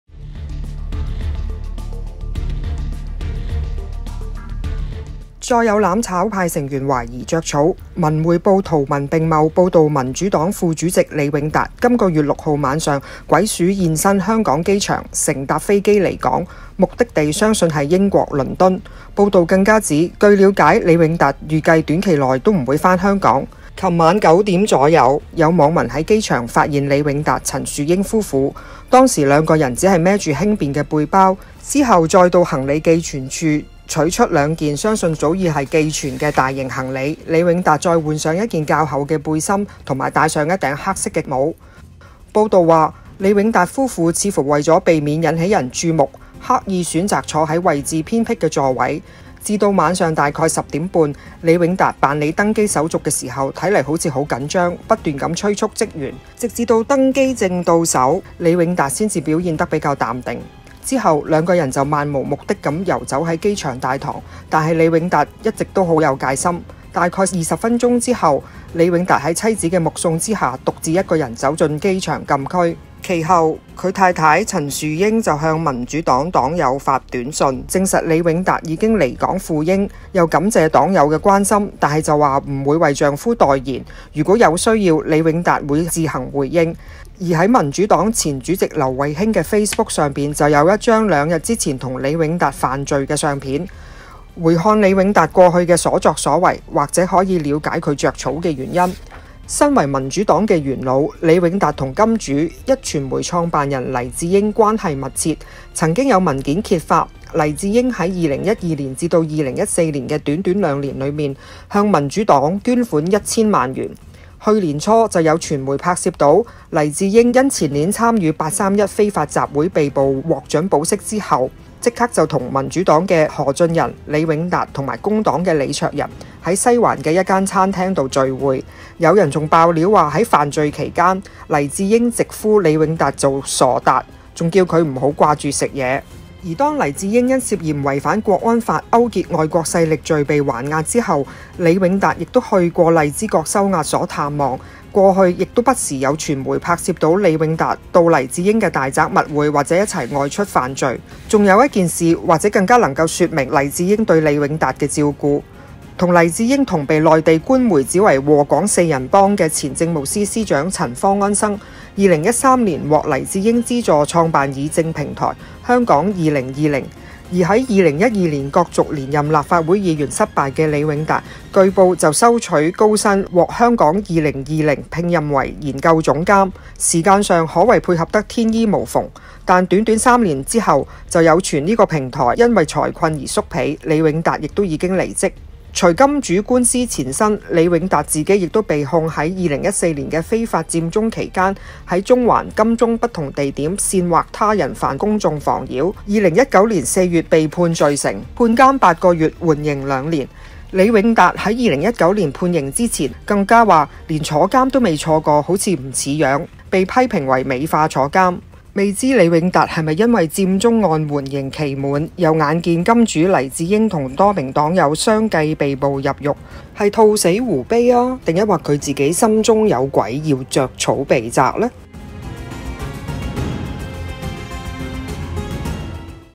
【短片】【焦點‧踢爆】「黎智英心腹」李永達 驚到震唔「著草」唔得?
//李永達6月當選民主黨副主席時，仲話要穩定軍心，依家卻疑似率先「著草」，攬炒派嘅「齊上齊落」真係信一成都死//